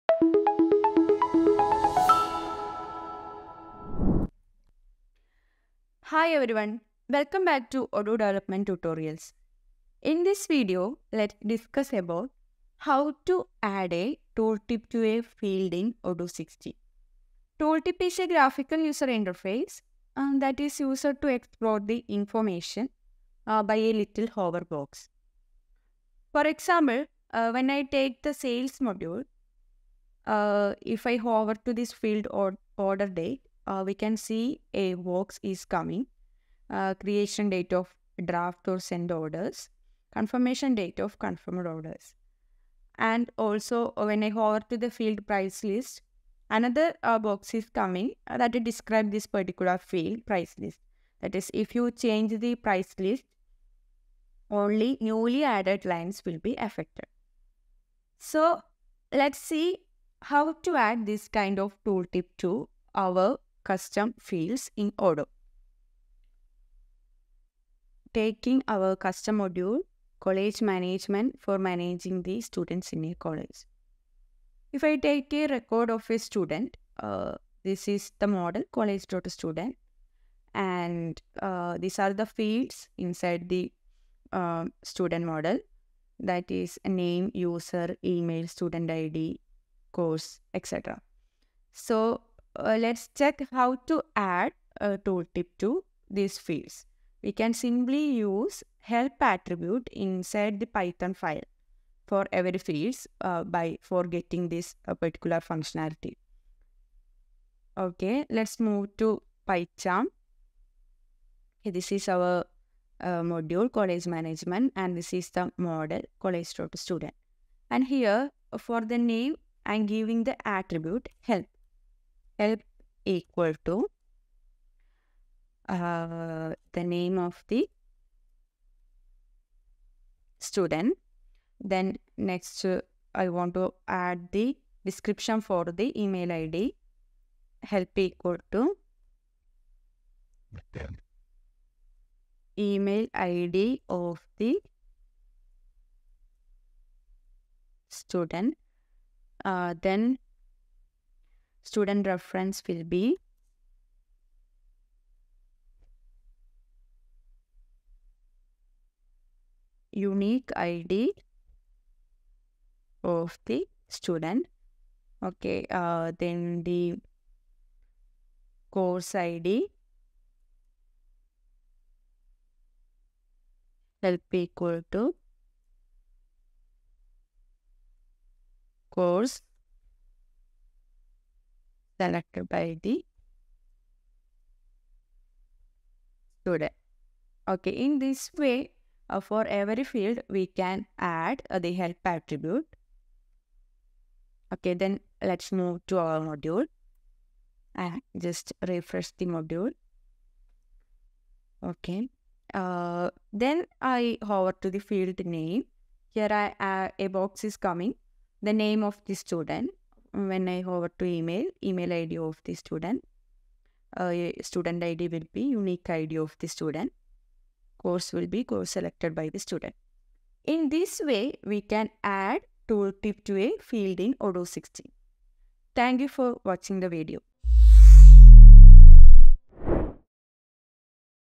How Add a Tooltip to a Field in Odoo 16 | Odoo 16 Development Tutorial
Help is the tooltip text shown when you hover the pointer over the field, and it allows you to override the help text provided by the model definition. In this video, we discuss how to add a tooltip to fields.